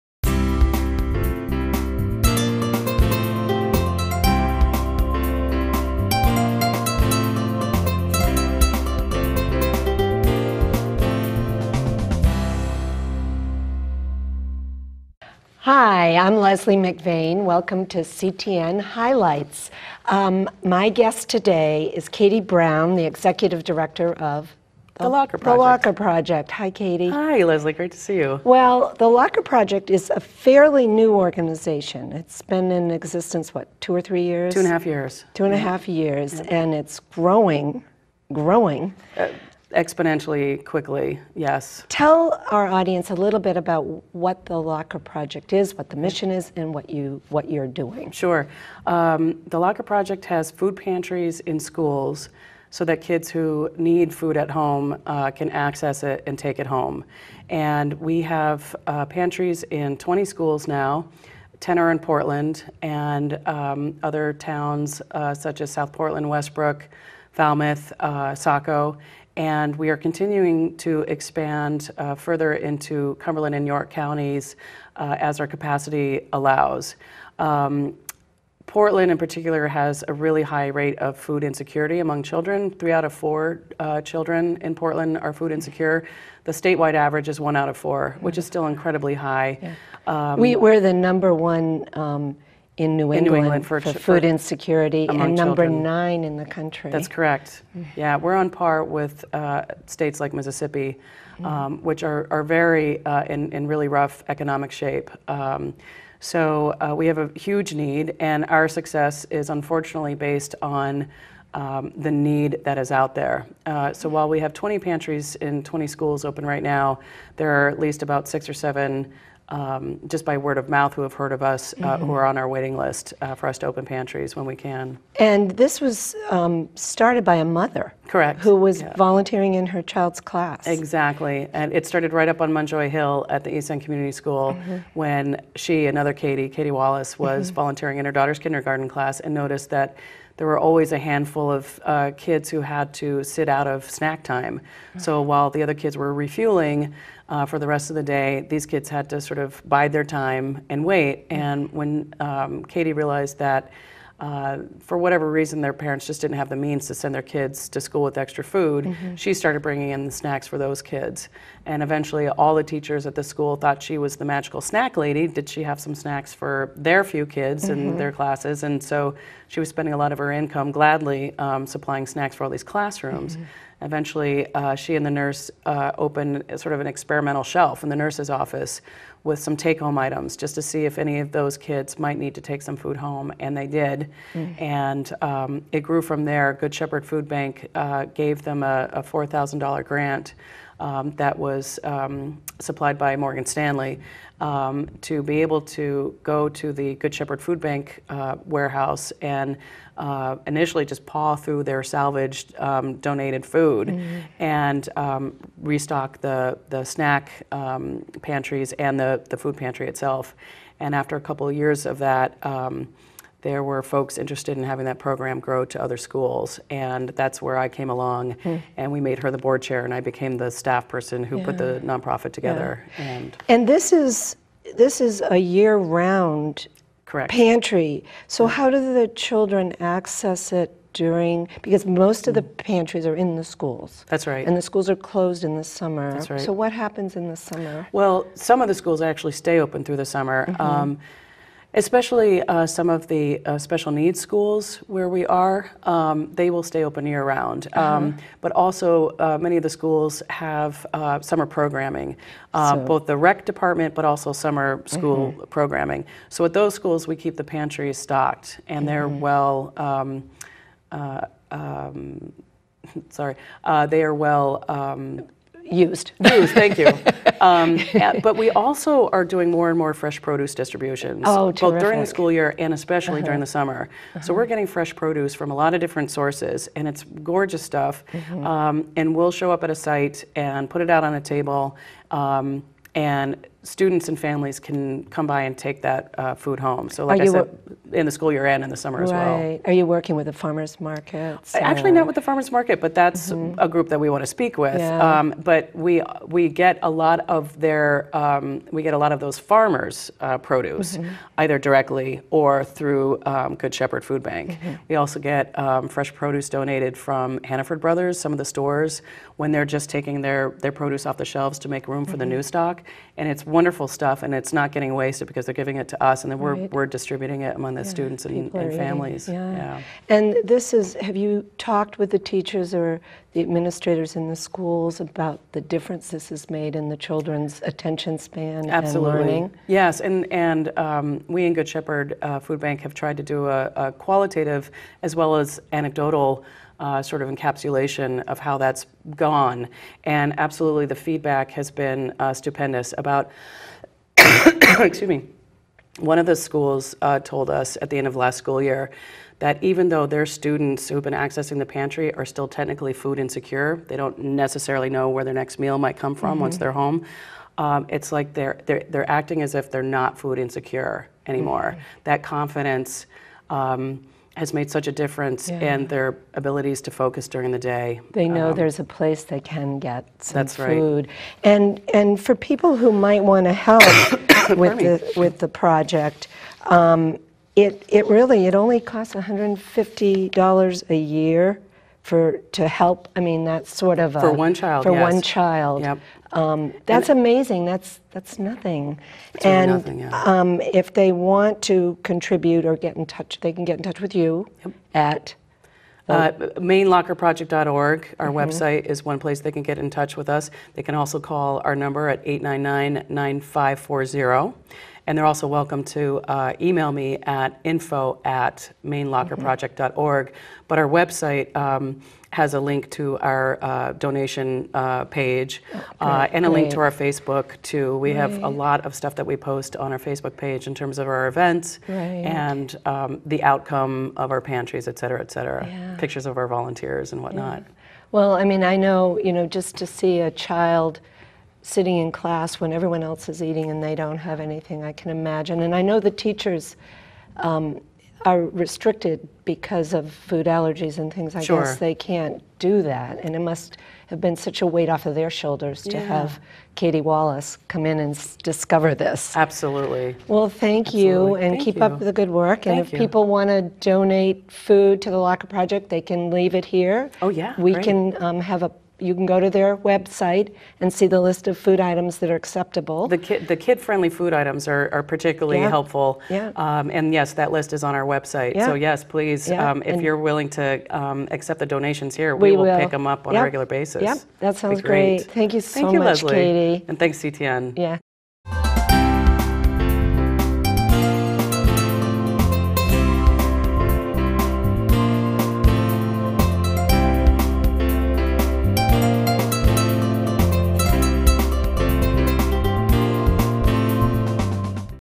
CTN5 - The Locker Project
Find out how The Locker Project connects food-insecure children in Maine with nourishing food to improve their learning capacity, health, and future.